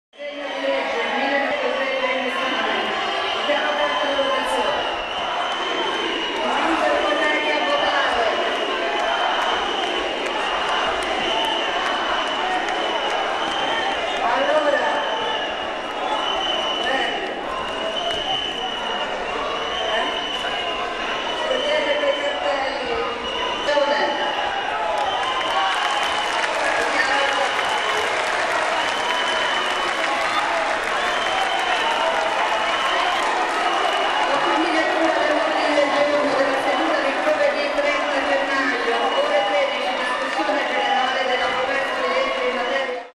Scuffles break out in Italian parliament
A fight broke out between MPs in Italy's lower house of parliament after the speaker interrupted a...

A fight broke out between MPs in Italy's lower house of parliament after the speaker interrupted a filibuster by opposition party M5S.